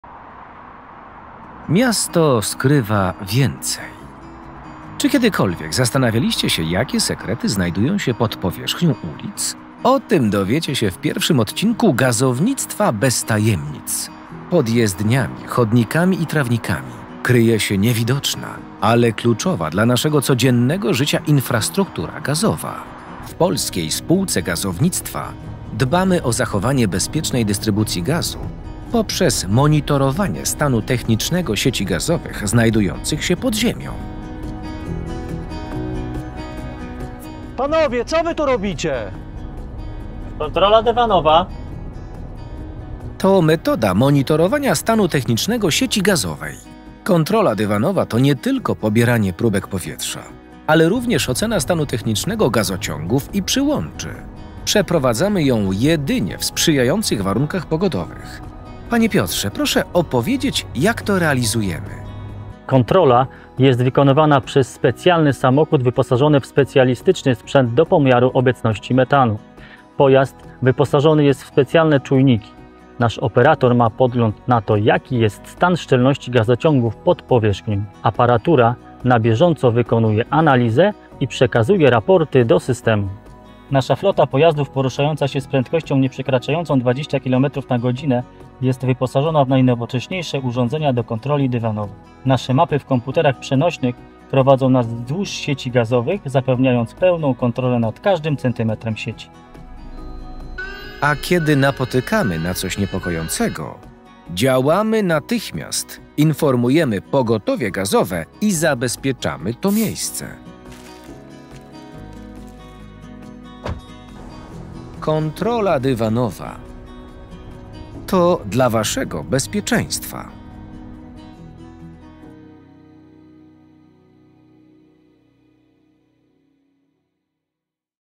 Kontrola dywanowa
Miasto skrywa więcej… Pod powierzchnią ulic i budynków, kryją się historie i niezwykłe sekrety. Czy znasz wszystkie zakamarki miasta? Zastanawiałeś się co skrywają podziemia miejskich ulic?

O tym dowiecie się w pierwszym odcinku „Gazownictwa bez tajemnic”.
Czym jest kontrola dywanowa?

To innowacyjna metoda do monitorowania stanu technicznego sieci gazowej! Kontrola dywanowa to nie tylko pobieranie próbek powietrza,
ale również ocena stanu technicznego gazociągów i przyłączy.
Zachęcamy do obejrzenia filmu.